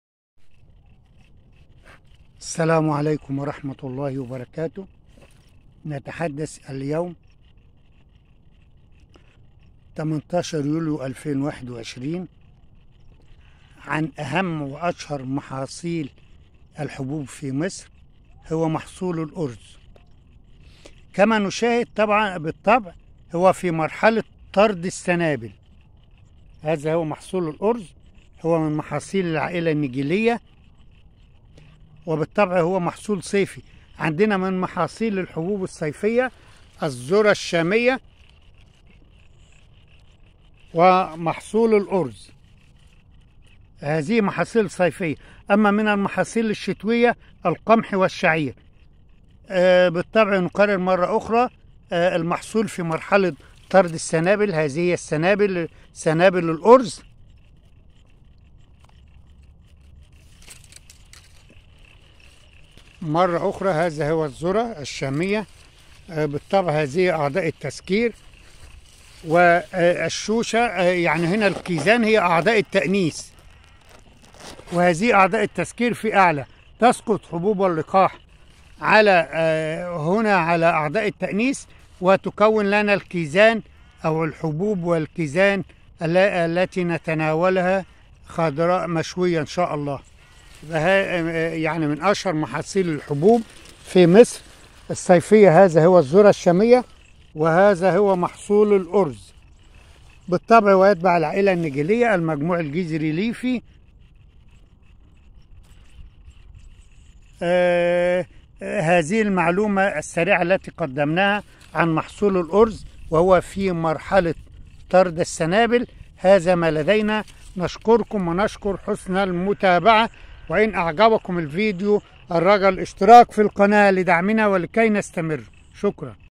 محاصيل الحبوب الصيفية ،
البقلية ،من محاصيل الحبوب الصيفية ،( الذرة الشامية ، والأرز ) شاهد الارز في مرحلة طرد السنابل ، والذرة  نبات ، توجد أعضاء التذكير في اعلي النبات ﴿ الشوشة﴾. وعضو التأنيث الكوز